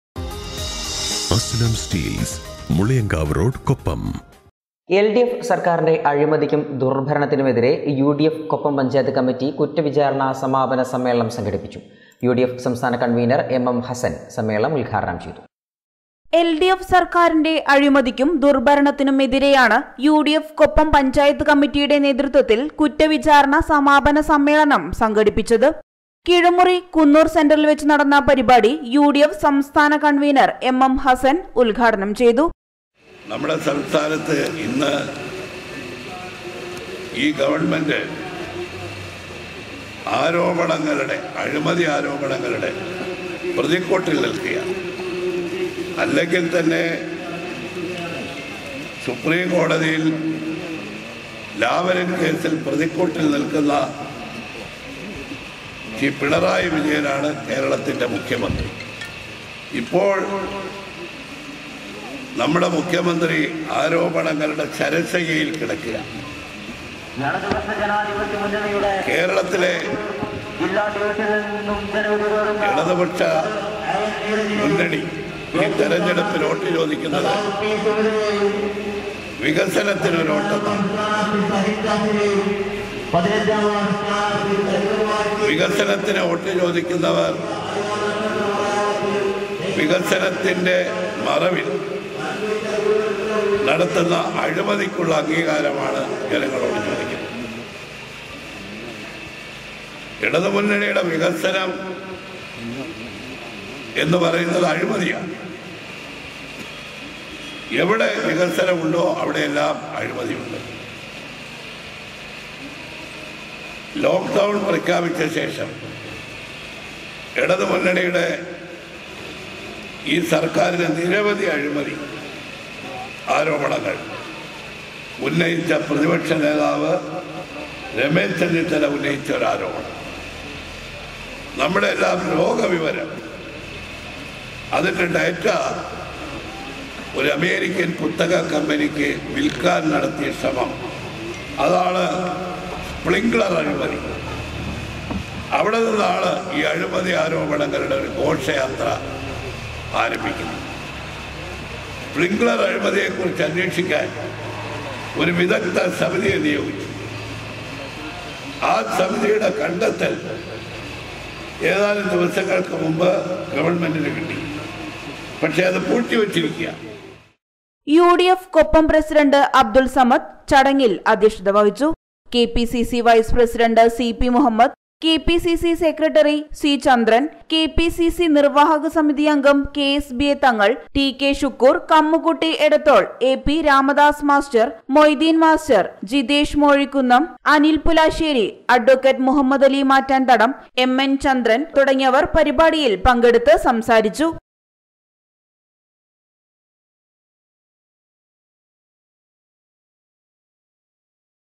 UDF കൊപ്പം പഞ്ചായത്ത് കമ്മറ്റി കുറ്റവിചാരണ സമാപന സമ്മേളനം സംഘടിപ്പിച്ചു
LDF സർക്കാരിന്റെ അഴിമതിക്കും ദുർഭരണത്തിനും എതിരെ UDF കൊപ്പം പഞ്ചായത്ത് കമ്മറ്റി കുറ്റവിചാരണ സമാപന സമ്മേളനം സംഘടിപ്പിച്ചു. UDF സംസ്ഥാന കൺവീനർ എം.എം ഹസ്സൻ സമ്മേളനം ഉദ്ഘാടനം ചെയ്തു.